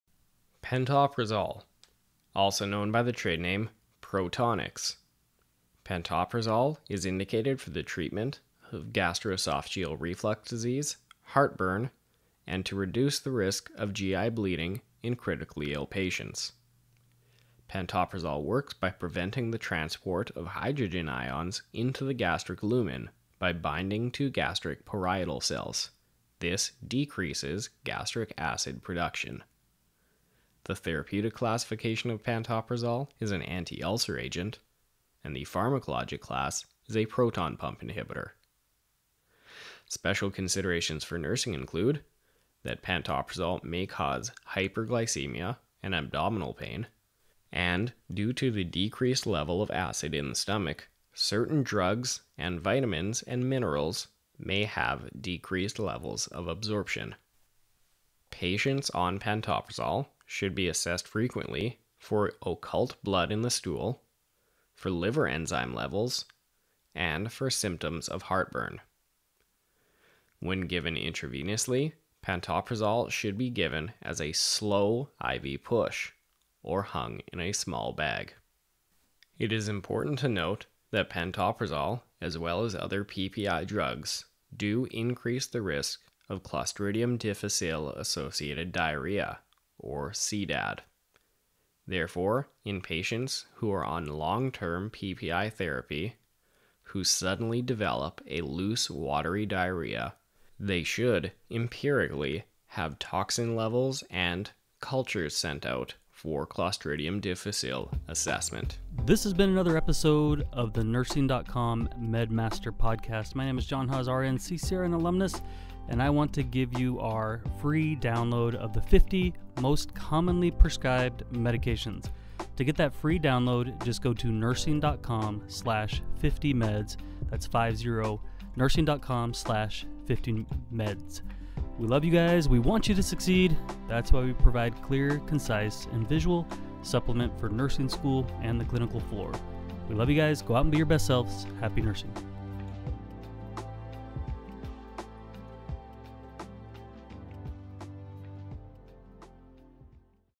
Pantoprazole (Protonix) Nursing Considerations
View the lesson:     Generic Name pantoprazole Trade Name Protonix Indication

GERD, heartburn, reduce the risk of GI bleed in critically ill patients
 Action

prevents the transport of H ions into the gastric lumen by binding to gastric parietal cells, ↓ gastric acid production
 Therapeutic Class

antiulcer agents
 Pharmacologic Class

proton-pump inhibitors
 Nursing Considerations

• can cause hyperglycemia, abdominal pain • decreases absorption of certain drugs • may increase bleeding with warfarin • assess for occult blood • assess liver enzymes • assess symptoms of heart burn • administer slowly via IV push